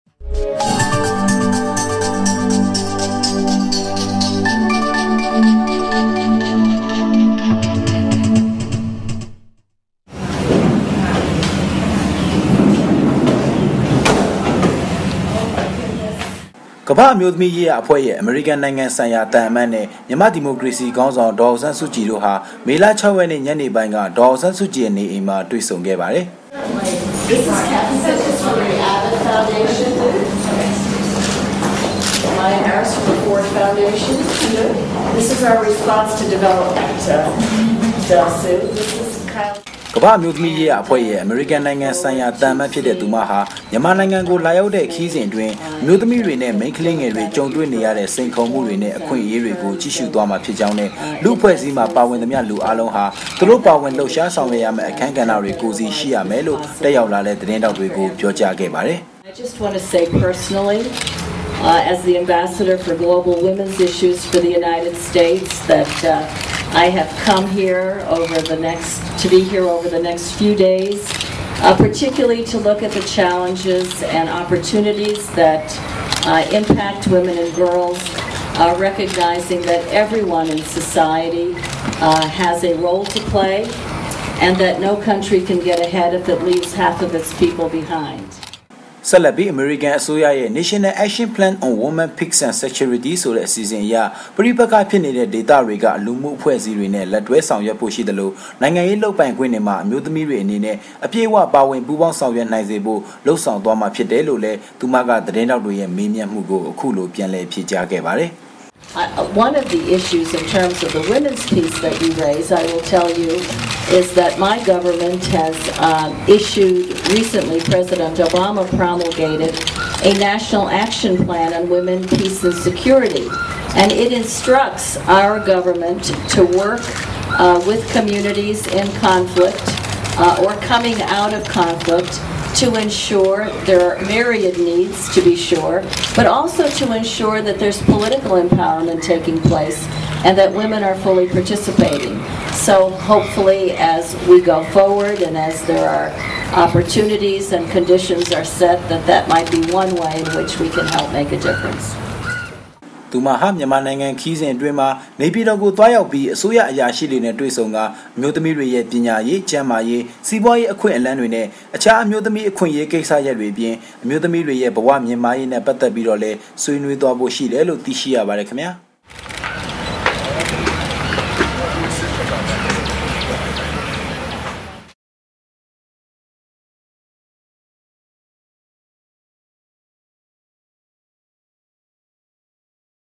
Aung San Suu Kyi meets with the Ambassador-at-Large for Global Women's Issues
ကမၻာ့အမ်ိဳးသမီးေရးရာအဖြဲ႕ရဲ႕ အေမရိကန္ႏိုင္ငံဆိုင္ရာ သံအမတ္ Melanne Verveer နဲ႕ ျမန္မာ့ဒီမိုကေရစီ ေခါင္းေဆာင္ေဒၚေအာင္ဆန္းစုၾကည္တို ့ဟာ ေမလ ၆ ရက္ေန ့ညေနပိုင္းက ေဒၚေအာင္ဆန္းစုၾကည္ရဲ႕ ေနအိမ္မွာ ေတြ႕ဆံုခဲ့ပါတယ္။ Melanne Verveer ဟာ ျမန္မာႏိုင္ငံကို လာေ၇ာက္တဲ့ ခရီးစဥ္အတြင္း အမ်ိဳးသမီးေတြနဲ႕ မိန္းကေလးငယ္ေတြ ၾကံဳေတြ႕ေနရတဲဲ့ စိန္ေခၚမႈေတြ နဲ႕ အခြင့္အေရးေတြကို ၾကည့္ရႈသြားမွာ ျဖစ္ေၾကာင္းနဲ႕ လူ ့အဖြဲ႕အစည္းထဲမွာ ပါဝင္သမွ် လူအားလံုးဟာ သူတို ့ ပါဝင္လႈပ္ရွား ေဆာင္ရြက္ရမယ့္ အခန္း႑ေတြ ကိုယ္စီ ရွိရမယ္လို ့ တက္ေရာက္လာတဲ့ သတင္းေထာက္ေတြကို ေျပာၾကားခဲ့သည္။